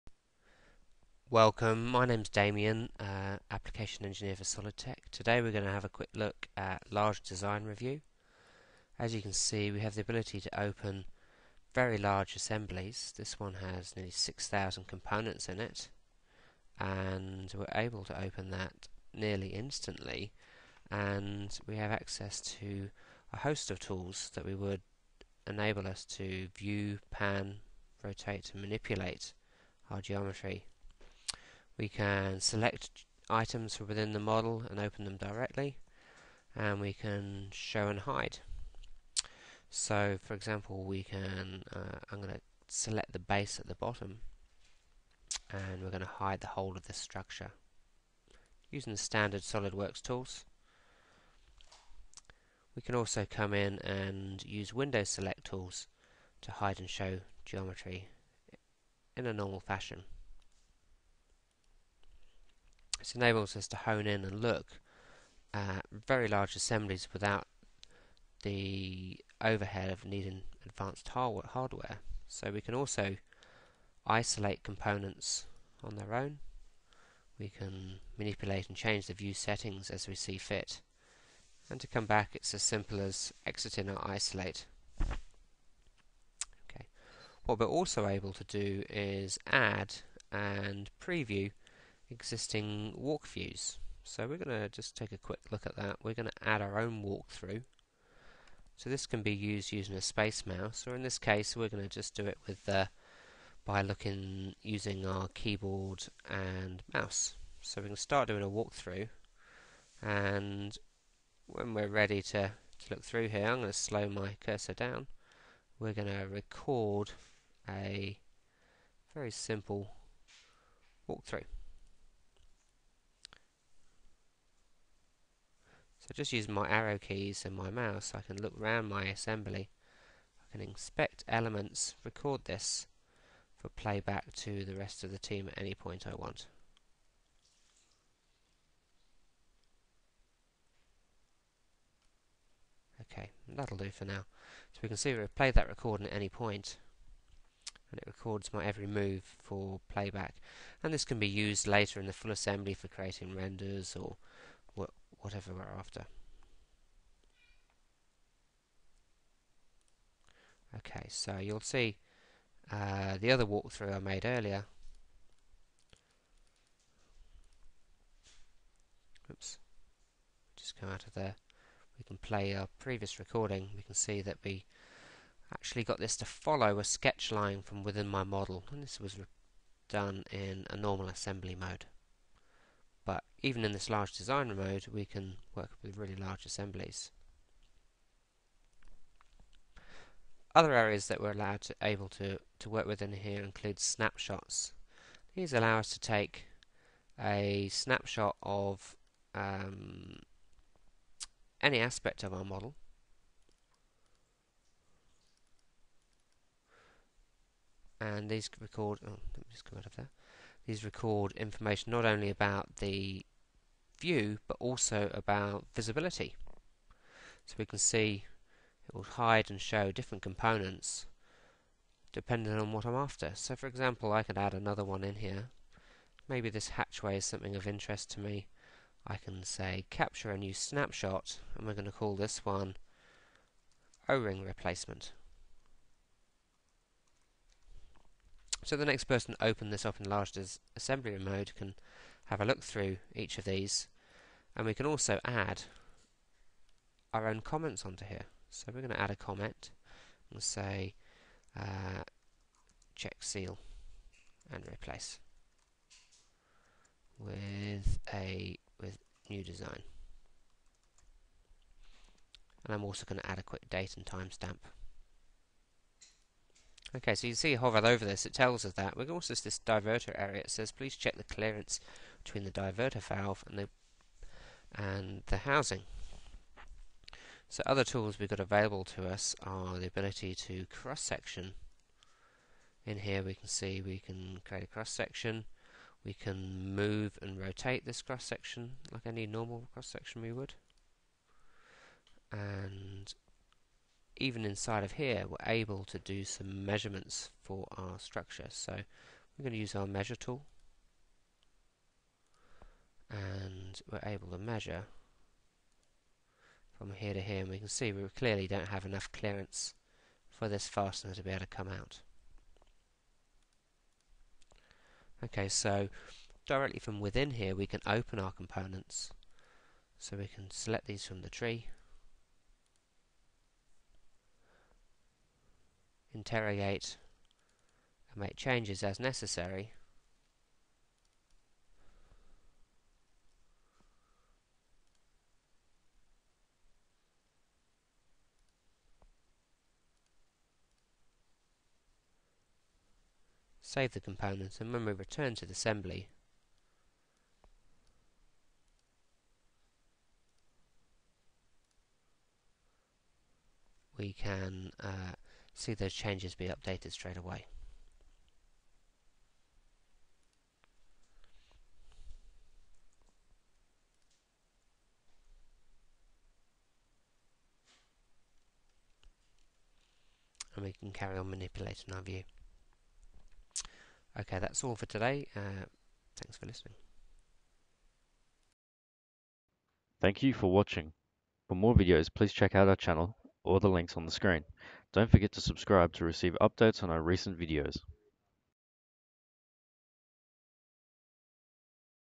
Large Design Review mode in SolidWorks
Applications Engineer Damien Murphy shows us the capabilities of SolidWorks when opening up large models, by utilizing the functionality/features available that assist in increasing the performance and decreasing unnecessary loading times.